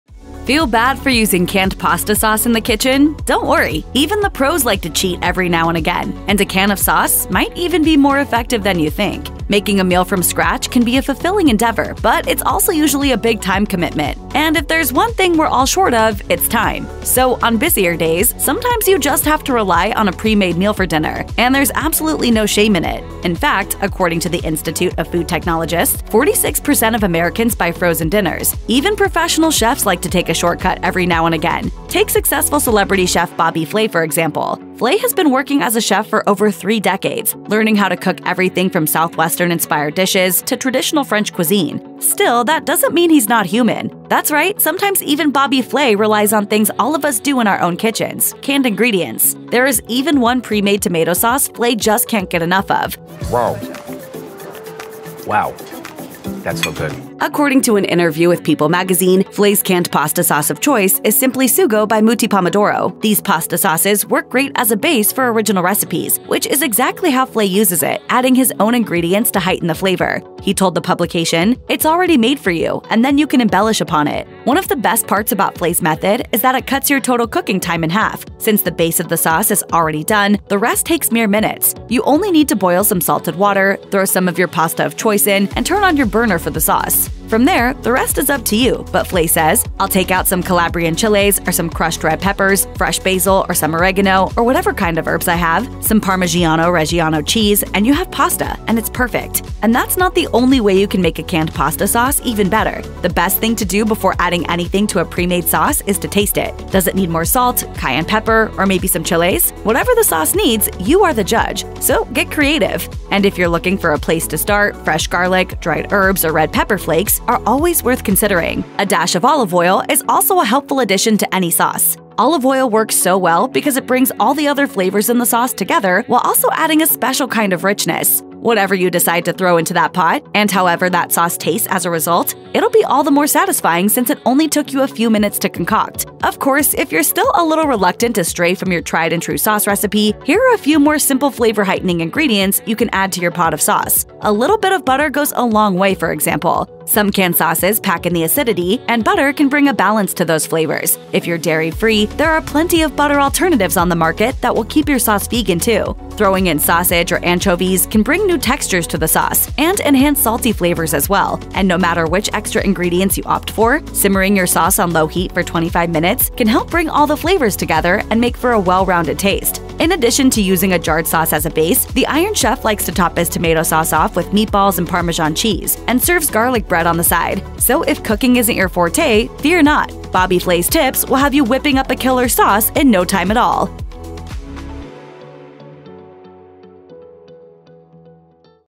Bobby Flay Reveals Which Store Bought Pasta Sauce He Uses
Feel bad for using canned pasta sauce in the kitchen? Don't worry — even the pros like to cheat every now and again. And a can of sauce might even be more effective than you think…